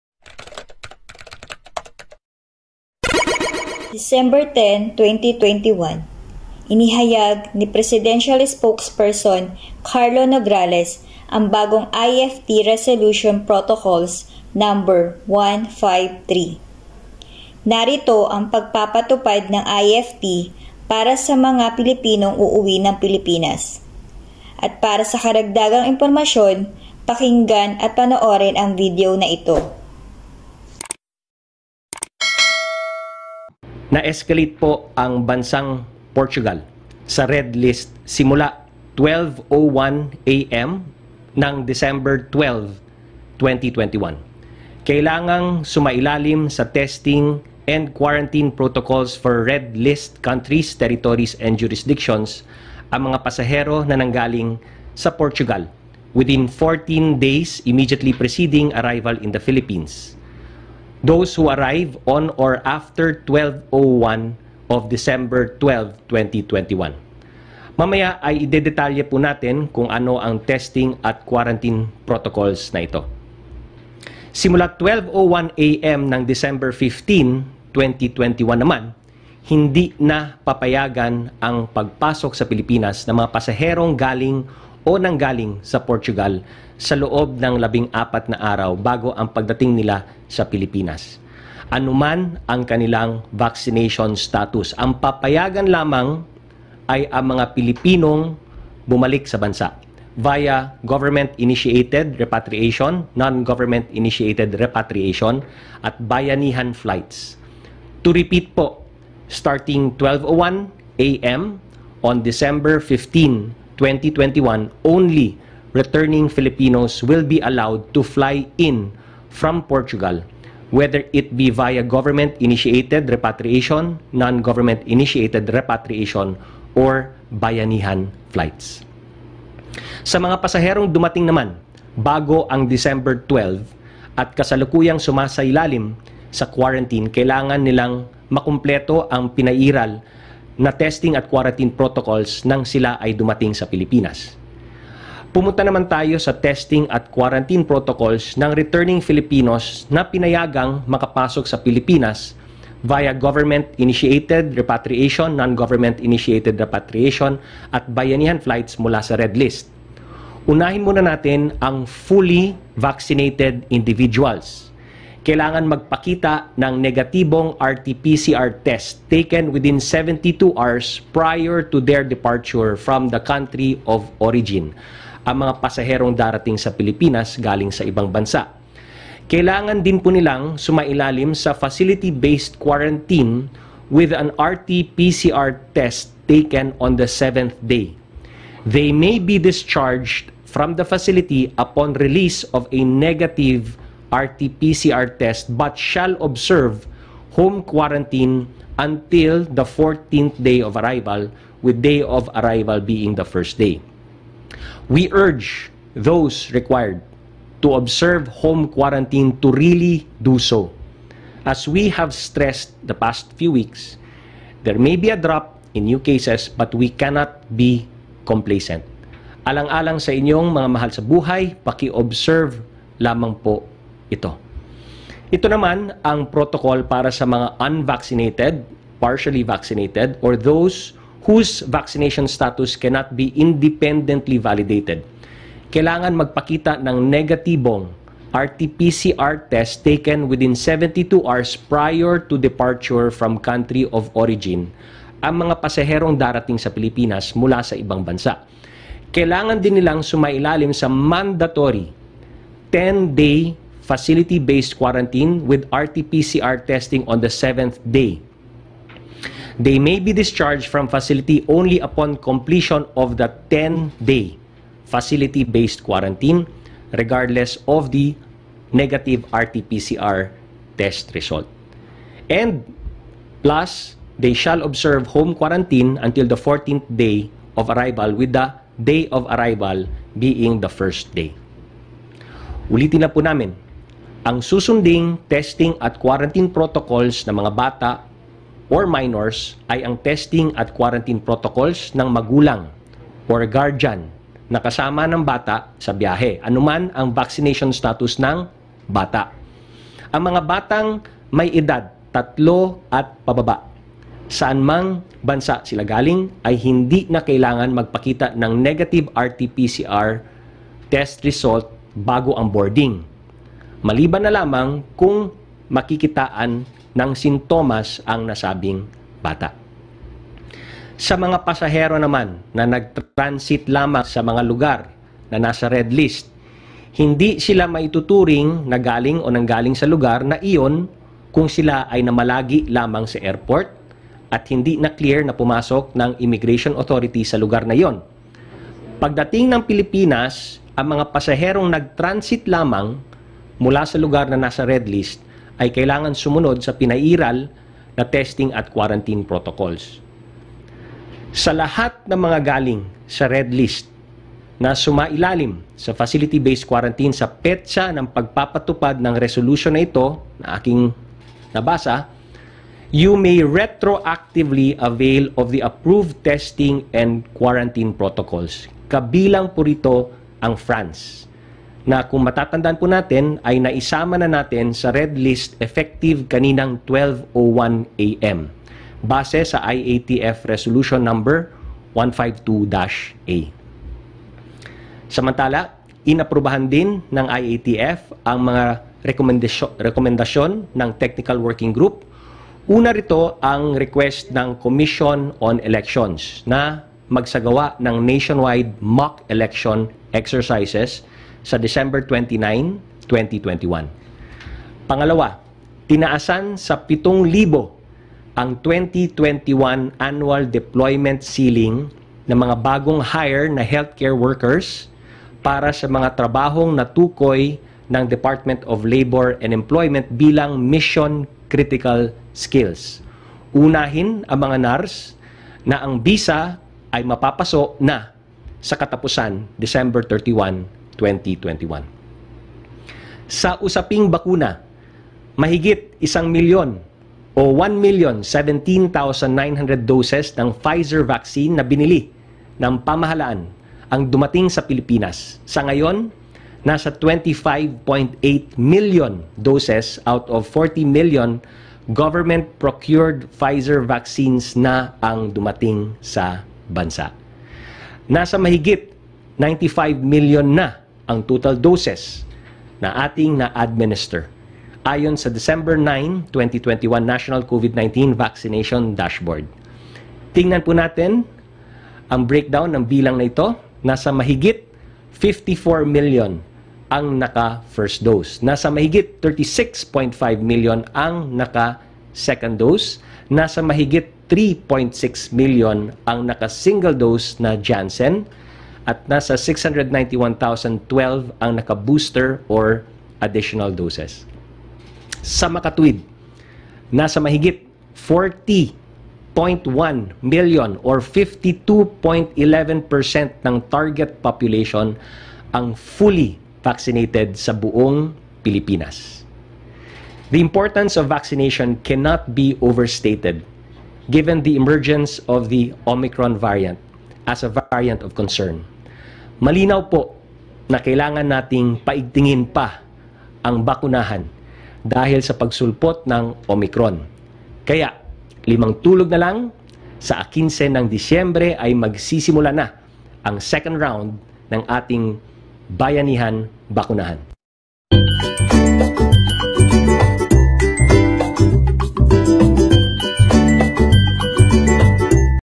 MULA SA BAGONG IAFT RESOLUTION... MAHALAGANG IMPORMASYON PARA SA MGA PILIPINONG UUWI NG PILIPINAS
Bagong anunsyo ng IAFT ngayong DEC. 10, 2021, ANG MAHALAGANG IMPORMASYON PARA SA mga PILIPINONG UUWI NG PILIPINAS.